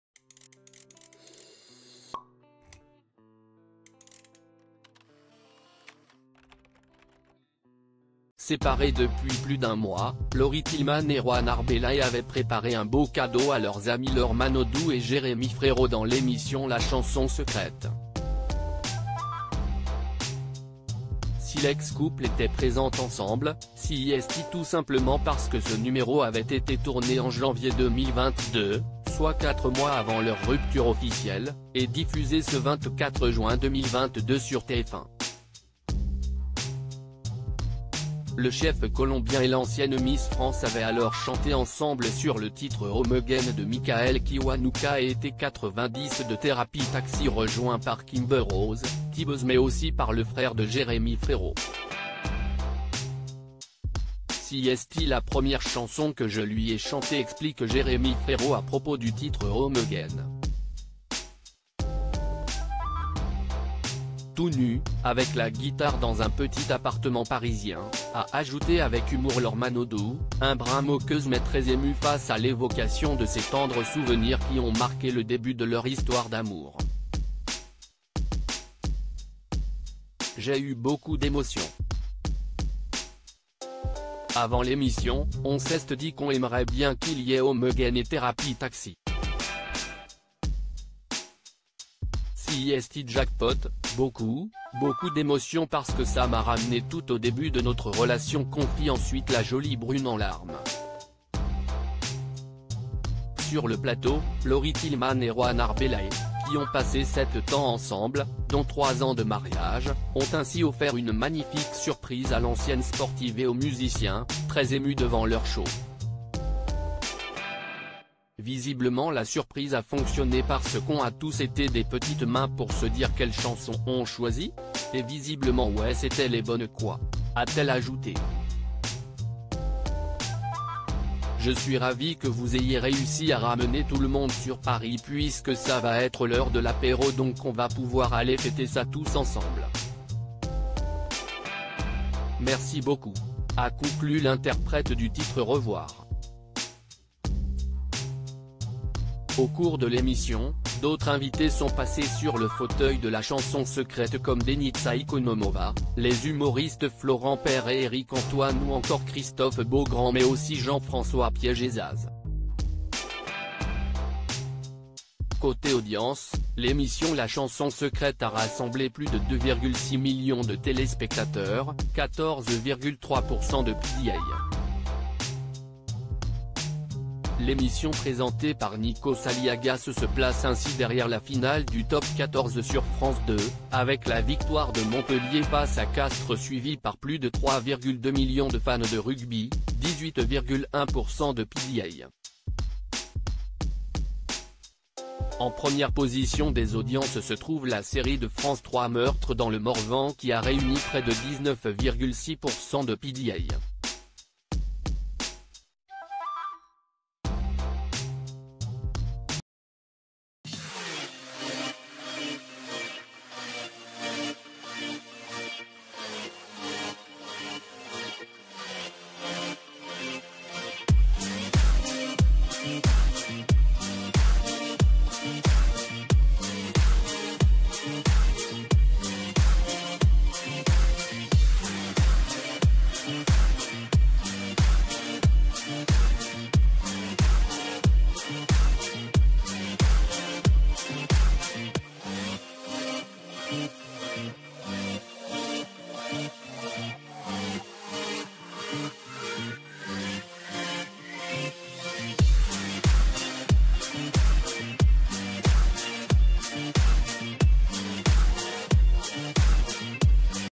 Laury Thilleman et son ex Juan Arbelaez réunis dans la Chanson Secrète : la présence du couple expli
Laury Thilleman et son ex Juan Arbelaez réunis dans la Chanson Secrète : la présence du couple expliquée